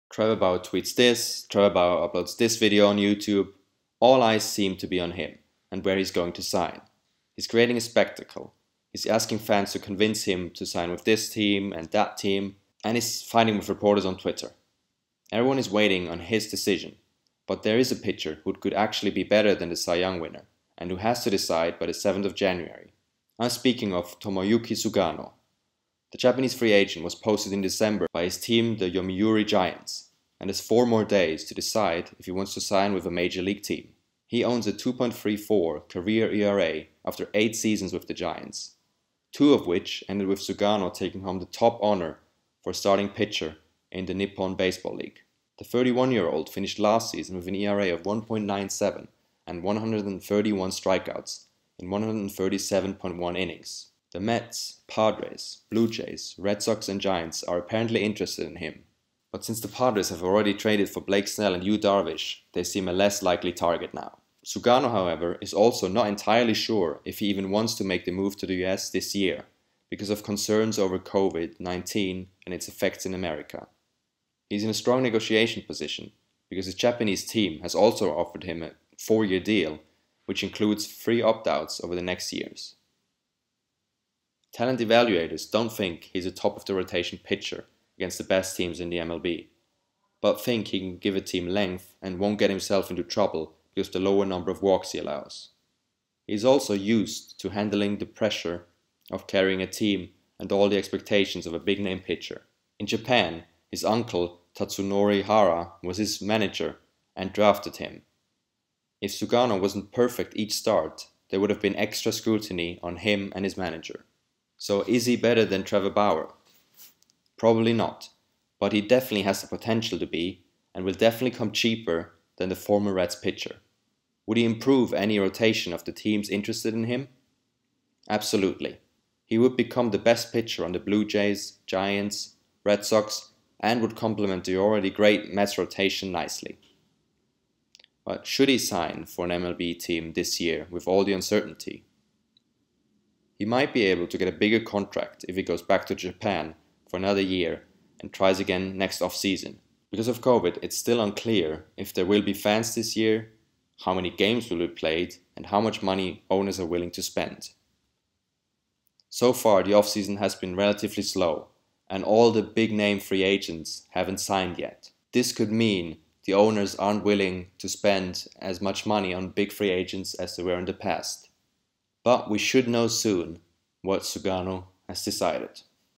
Is this Free Agent better than Trevor Bauer?
Japanese pitcher Tomoyuki Sugano has until the seventh of January to sign for an MLB team. Who is the 31-year old who dominated the Japanese league for years? Is he the best free agent pitcher on the market? Even better than Trevor Bauer and will he actually sign?  In this video I will answer some of the important questions.